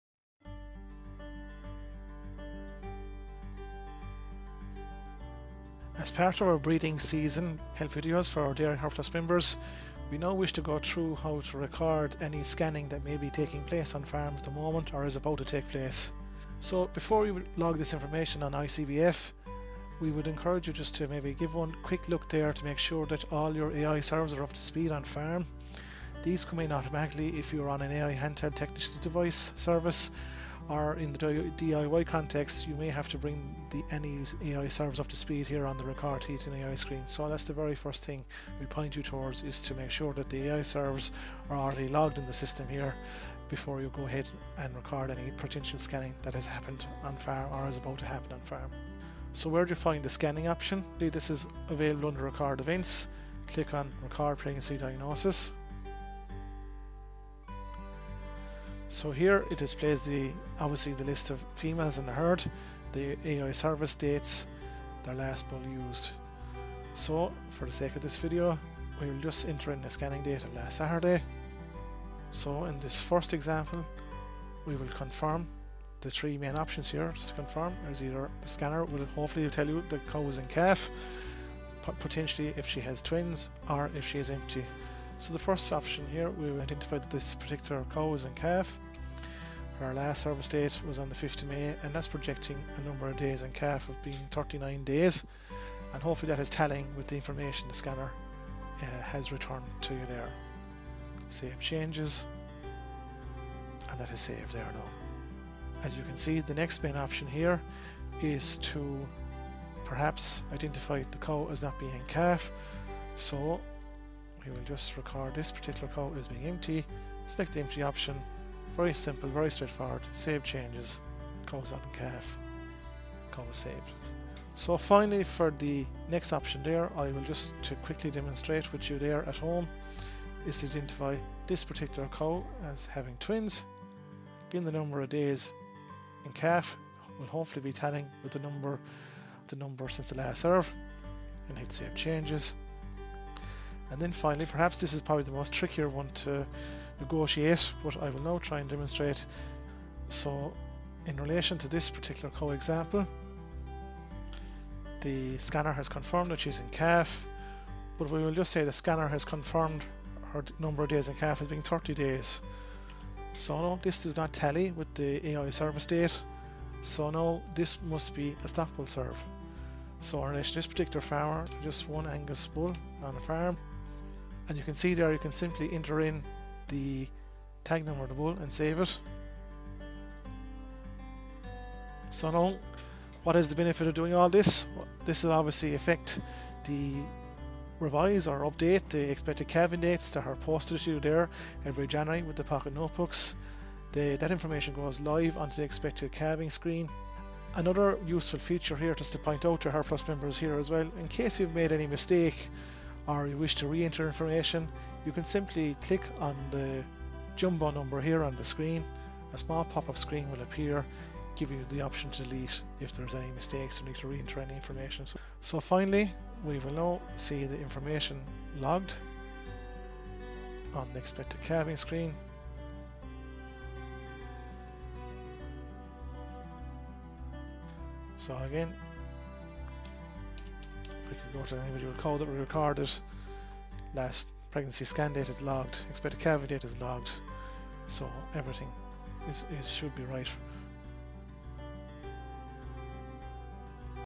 Recording Scanning Results on ICBF.com
ICBF's Dan O Riordan talks through how to record your scanning results on icbf.com